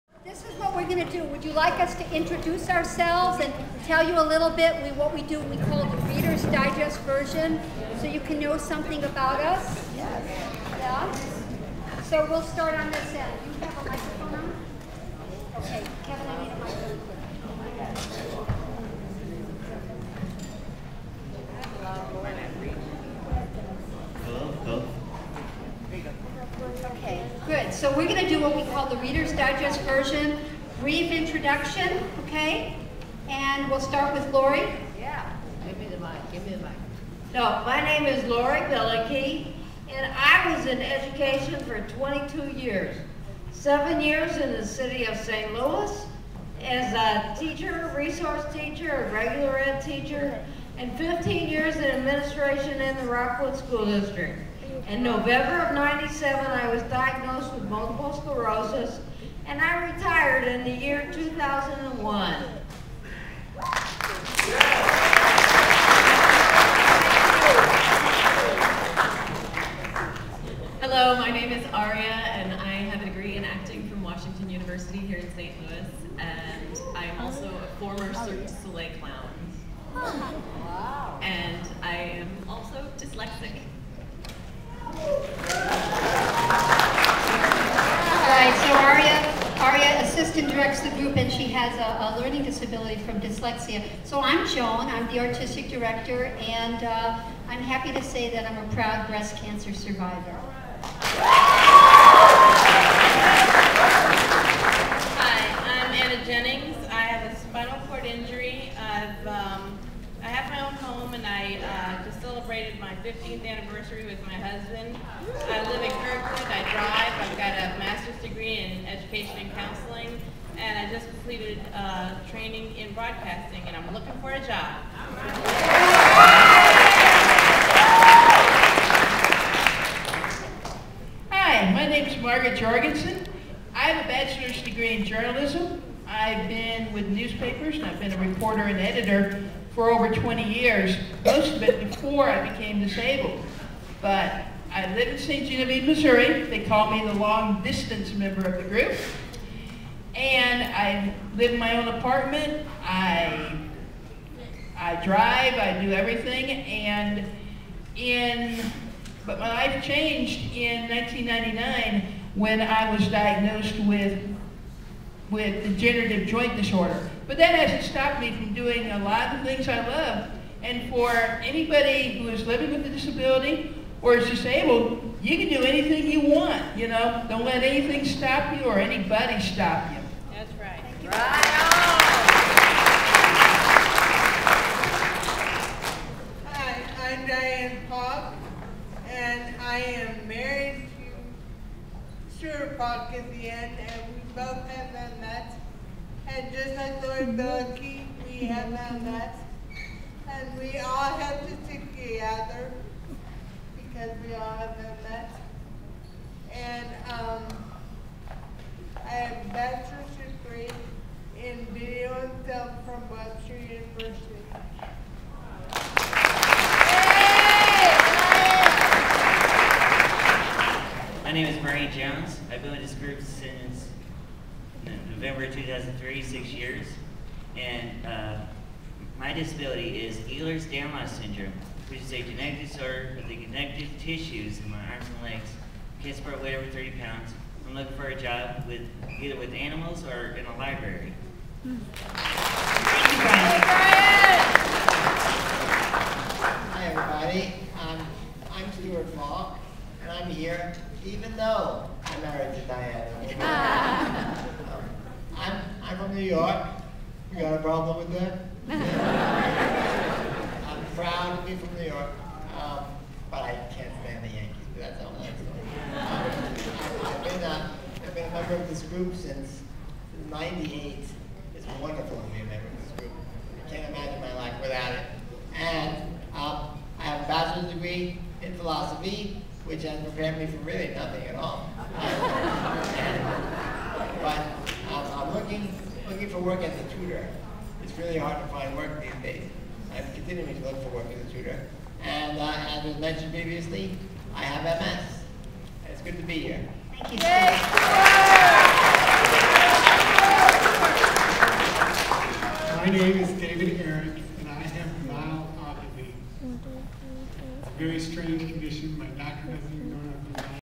The DisAbility Project: Hello
Members of The DisAbility Project, a touring theatrical ensemble based in St. Louis, share their stories with an audience of schoolchildren during a performance in the fall of 2009. Some members of the group even use the stage to pitch themselves for a new job. Read more about members of this group and their cause in the Riverfront Times